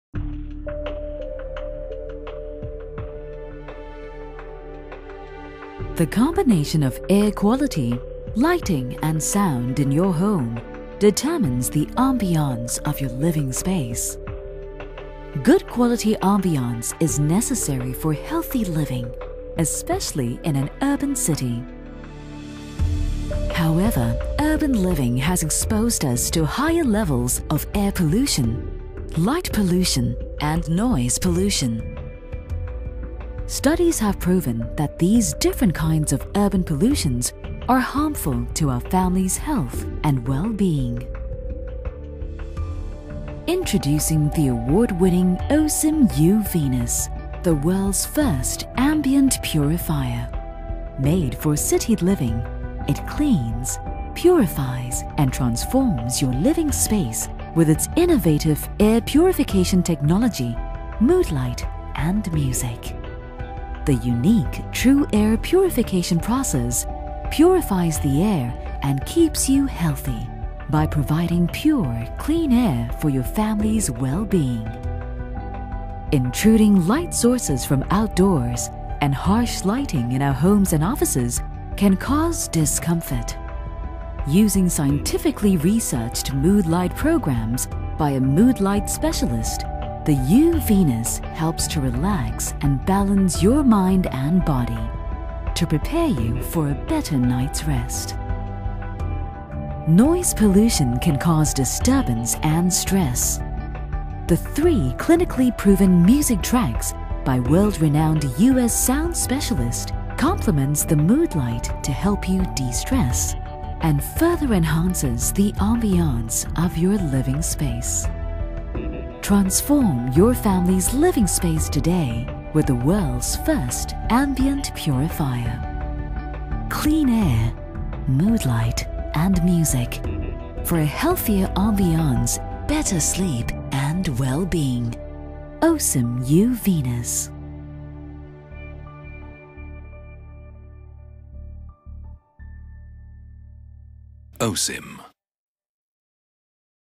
OSIM uVenus: Protect your family from air pollution, light pollution & noise pollution
OSIM uVenus - The world's 1st Ambient Purifier

Air Pollution is prevalent both outdoors and indoors. These invisible pollutants accumulate in our living space, weakening our body's immune system and are often the main cause of allergies and respiratory ailments.

The uVenus with its Multi-action Tru-air purification process purifies stale indoor air and provides clean, fresh air for easier breathing and better well-being for your family.